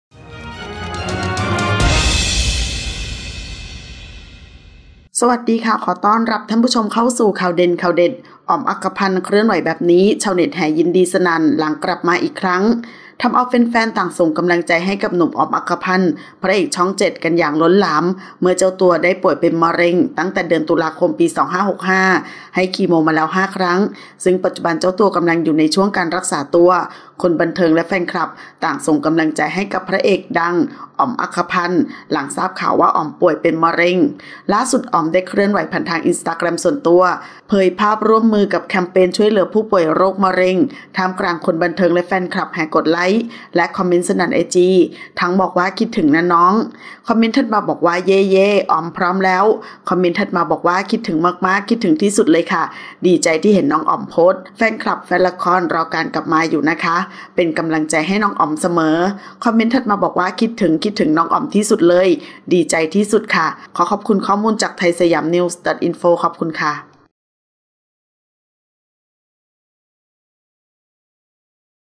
‘อ๋อม อรรคพันธ์’ เคลื่อนไหวแบบนี้ ชาวเน็ตแห่ยินดีสนั่นหลังกลับมาอีกครั้ง
ทำเอาแฟนๆต่างส่งกำลังใจให้กับหนุ่ม “อ๋อม อรรคพันธ์” พระเอกช่อง 7 กันอย่างล้นหลาม เมื่อเจ้าตัว
ได้ป่วยเป็นโรคมะเร็งตั้งแต่ตุลาคม 2565 ให้คีโมมาแล้ว 5 ครั้ง ซึ่งปัจจุบันเจ้าตัวกำลังอยู่ในช่วงการ

รักษาตัว  คนบันเทิงและแฟนคลับต่างส่งกำลังใจให้กับพระเอกชื่อดัง “อ๋อม-อรรคพันธ์ นะมาตร์”
หลังทราบข่าวกันว่าหนุ่มอ๋อมป่วยด้วยโรคมะเร็งบริเวณกล้ามเนื้อหัวใจ ได้รับการผ่าตัดเป็นที่เรียบร้อยแล้ว

ล่าสุด หนุ่มอ๋อมได้เคลื่อนไหวอินสตราแกรมส่วนตัว @omakapan เผยภาพร่วมมือกับแคมเปญช่วยเหลือ
ผู้ป่วยโรคมะเร็ง ท่ามกลางคนบันเทิงและแฟนคลับแห่กดไลค์ และคอมเมนต์สนั่นไอจี ทั้งบอกว่า คิดถึงนะน้อง,

เย้ๆๆ…อ๋อมพร้อมแล้ว, คิดถึงมากๆๆๆคิดถึงที่สุดเลยค่ะ ดีใจที่เห็นน้องอ๋อมโพสต์ แฟนคลับแฟนละคร รอการ
กลับมาอยู่นะคะ เป็นกำลังใจให้เสมอค่ะ, คิดถึงๆน้องอ๋อมที่สุดค่ะ ดีใจที่สุดค่ะ น้องอ๋อม ลงไอจี วันนี้ วันดี

สุดๆๆค่ะ, คิดถึงมากค่ะ, อยากชมผลงานแล้วค่ะ, พี่อ๋อมกลับมาแล้ว, โอ้ยดีใจ! เห็นแล้วFCใจชื้นขึ้นมาเลยค่ะ
อย่าหายไปนานนะคะ FCคิดถึงมากกก, หายแล้วใช่ไหมน้องอ๋อม, ดีใจมาก, คิดถึง พระเอกในดวงใจ และอื่นๆ

00:00 - Intro
00:10 - News
01:00 - Minute